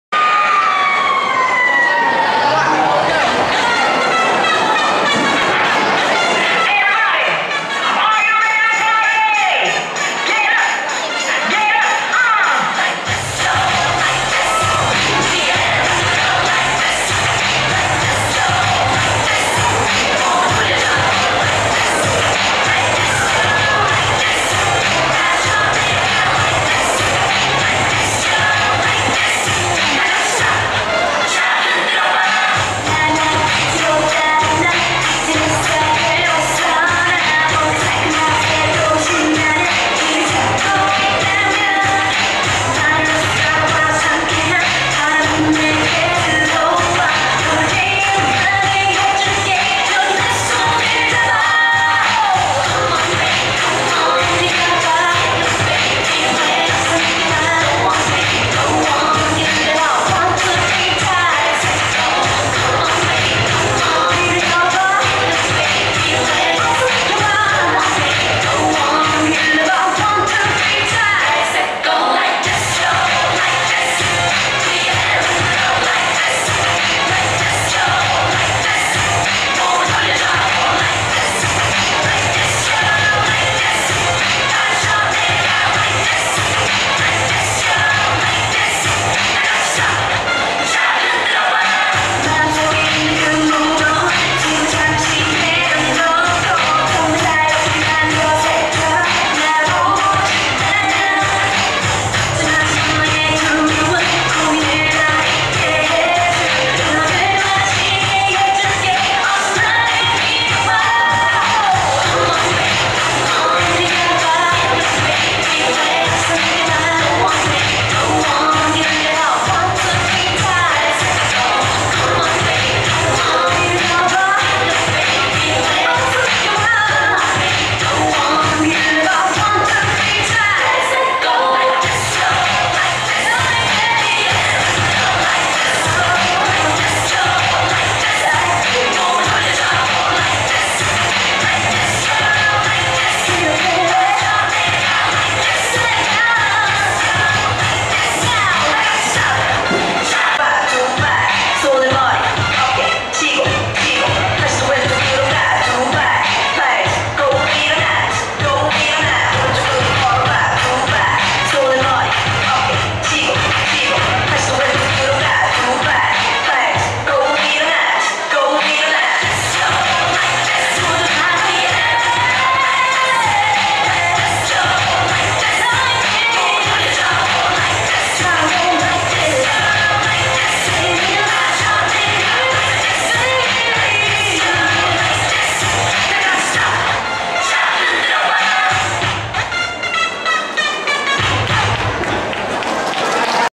[Khai giảng 2012-2013 DDT HN]Nhảy 12D1
Tiết mục: Nhảy hiện đại.
Biểu diễn: Lớp 12D1.
Khai giảng năm học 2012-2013.
Trường THPT Đào Duy Từ - Hà Nội.
4/9/2012.